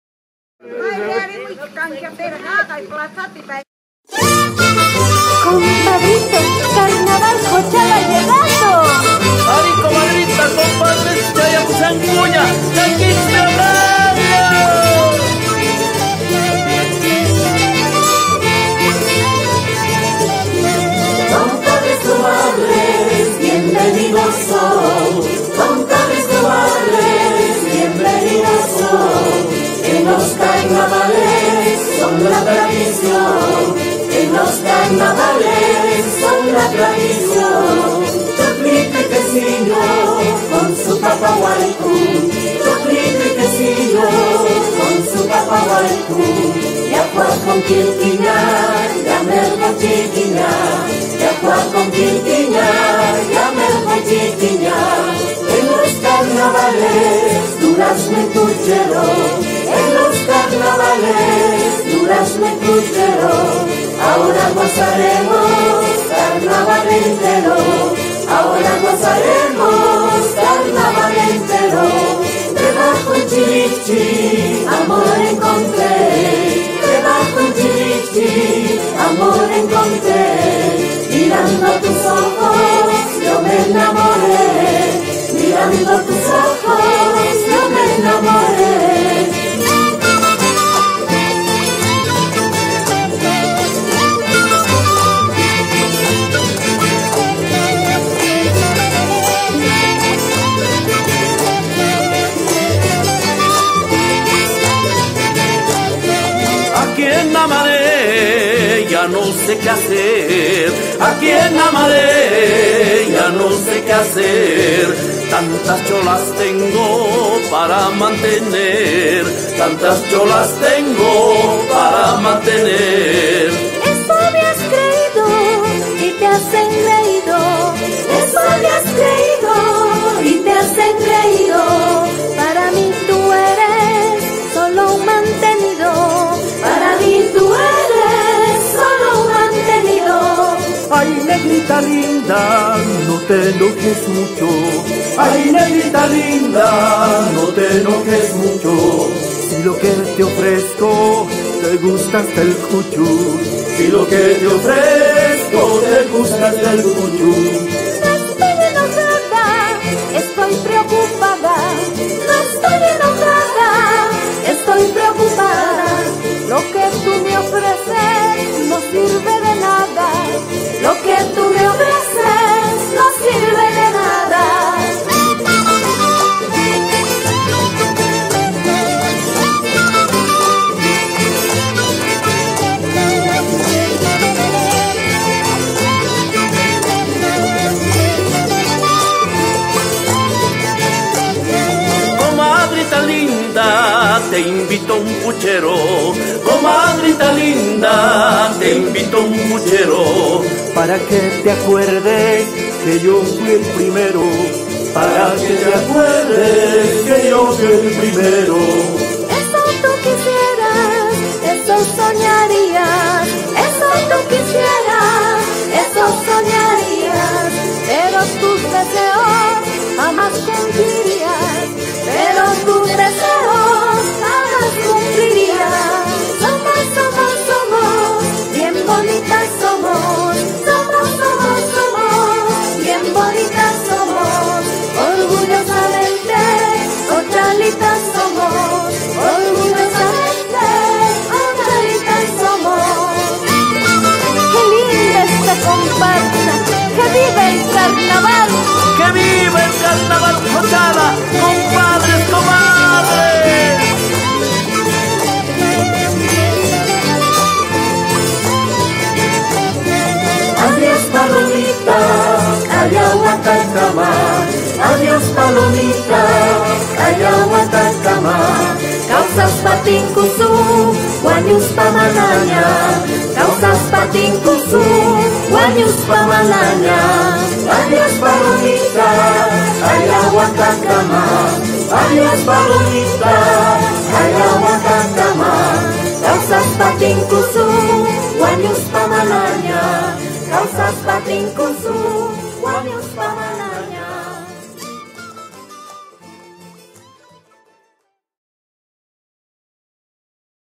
CARNAVAL COCHABAMBINO
Video realizado en la localidad de Pocoata, Arani Cochabamba.